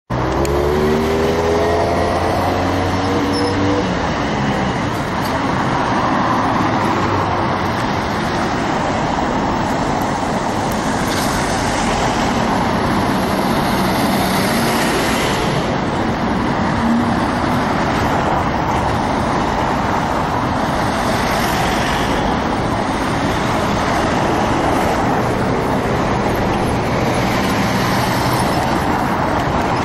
Convoi pompiers italiens aux champs Elysées, Paris
Passage d'un convoi de 5 véhicules des Sapeurs Pompiers italiens escorté par une VLR du SDIS 78 au champs Elysées à Paris (75).